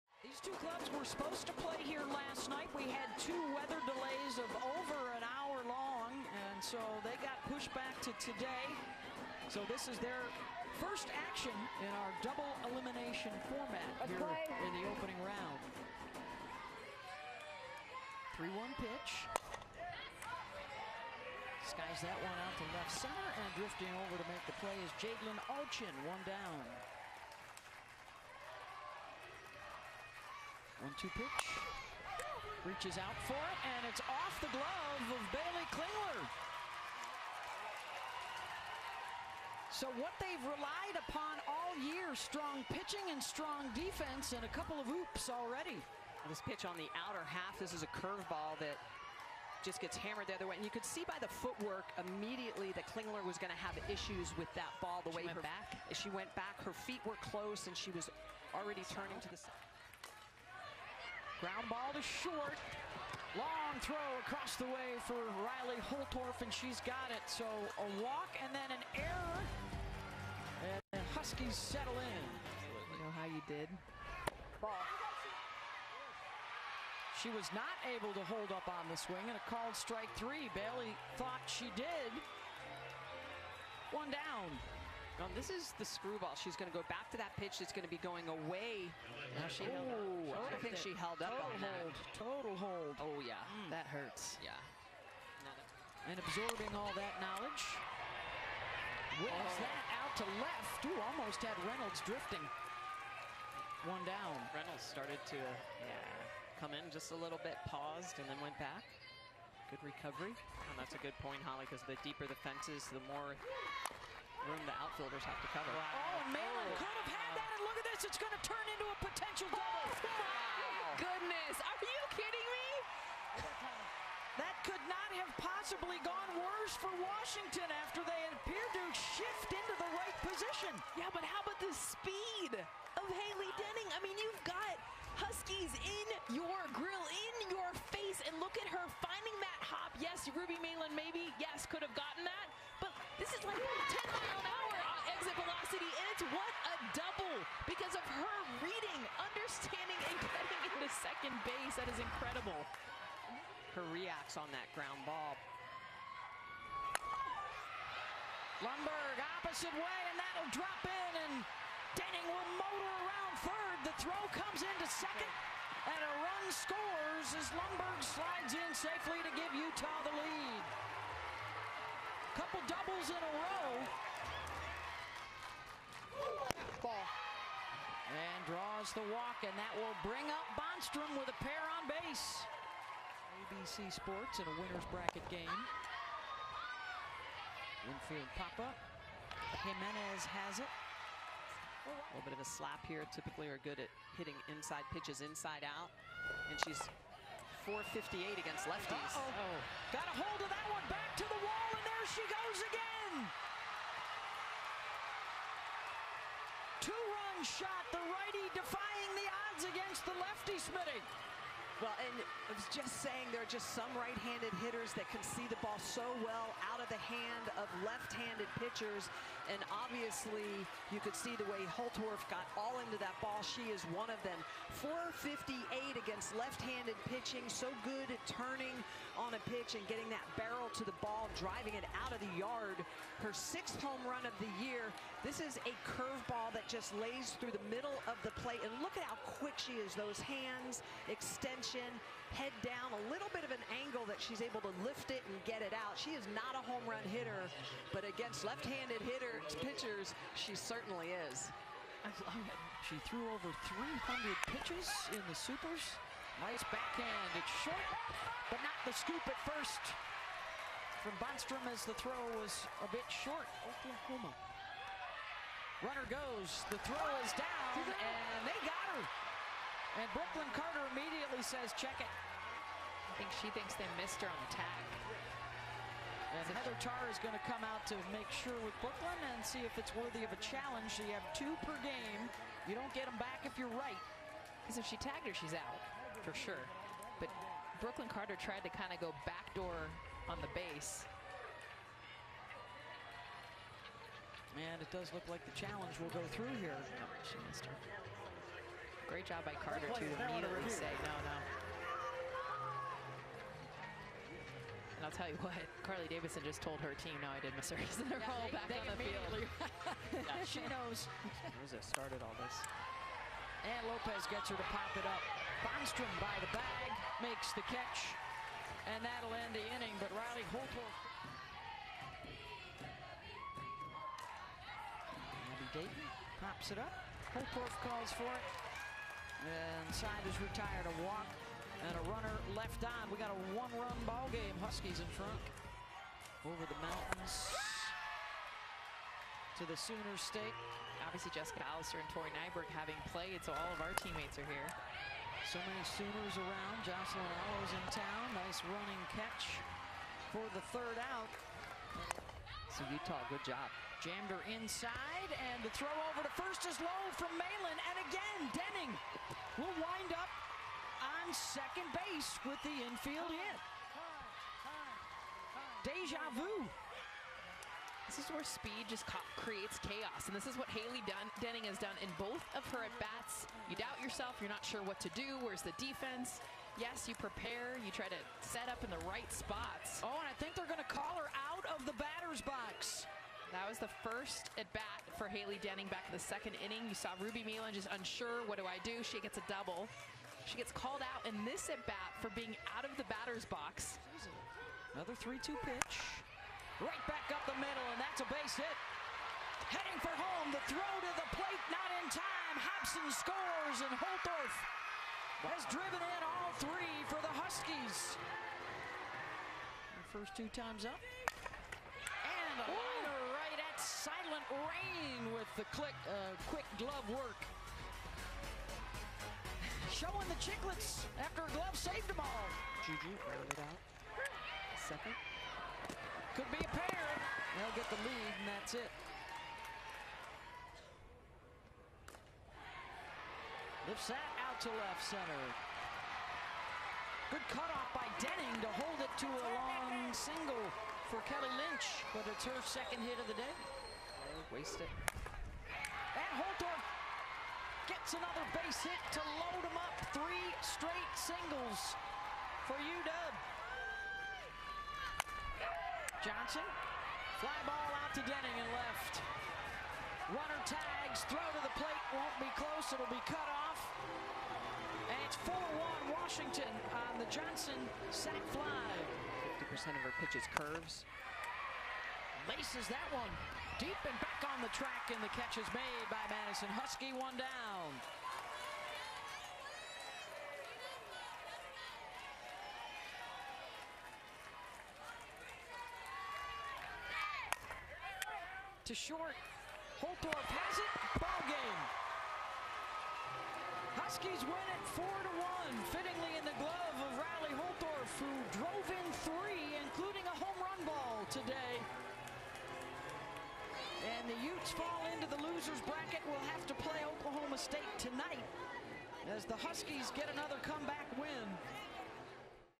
Washington vs. Utah: 2023 Women's College World Series highlights
Washington defeated Utah 4-1 in the opening round of the 2023 Women's College World Series. Watch the full highlights here.